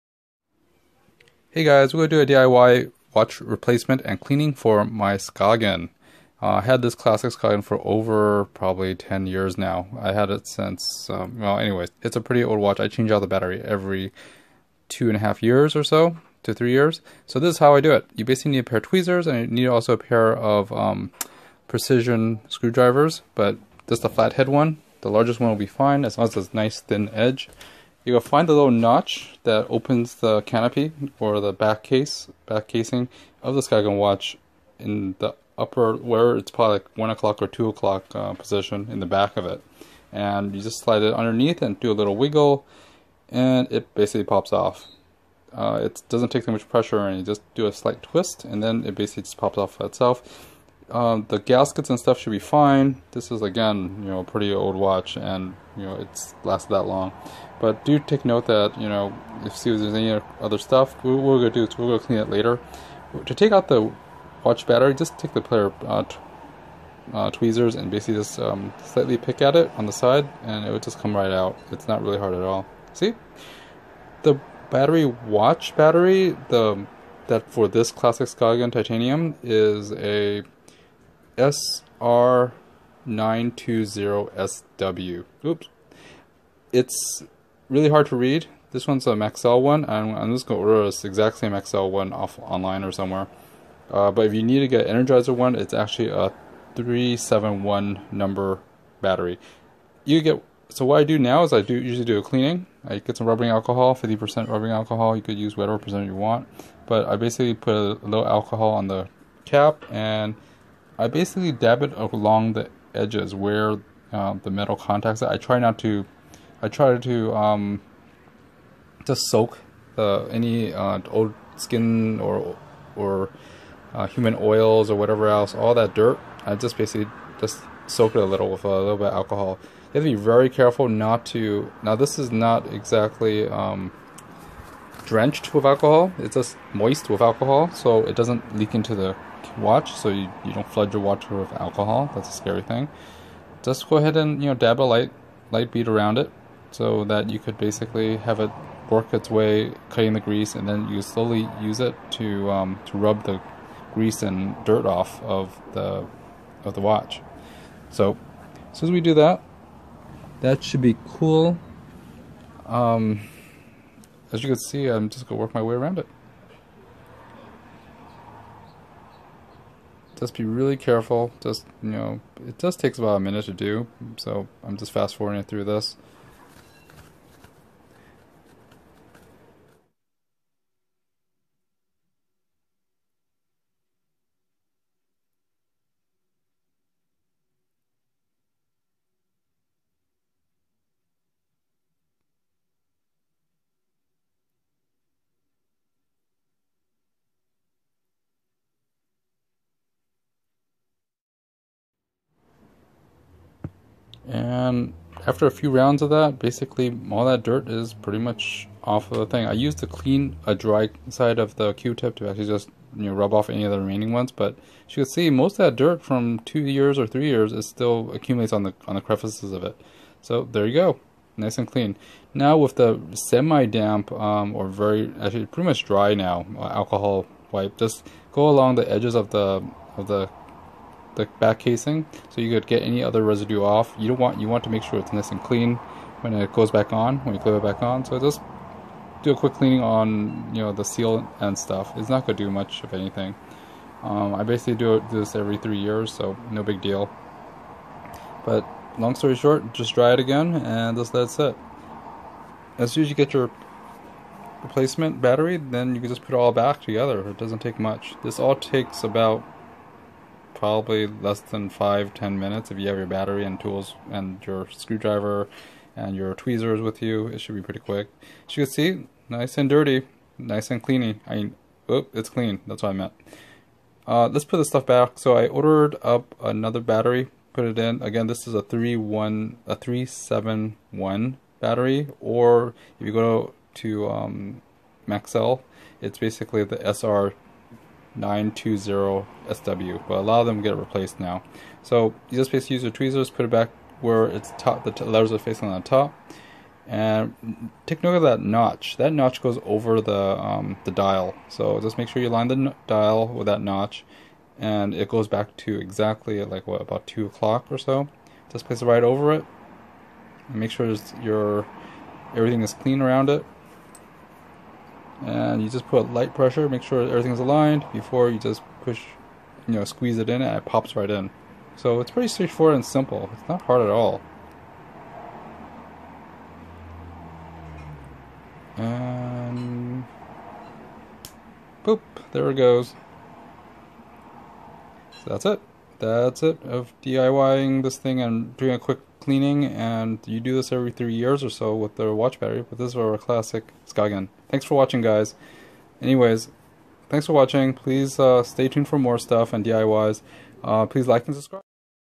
Watch battery replacement and cleaning Skagen
I replace and clean my Skagen watch about every 3 years. Here is a diy instructions with battery info below. Took about 10 minutes or less and a $4 watch battery battery.
Battery (SR920SW or #371)
Precision Tools
Skagen slim watches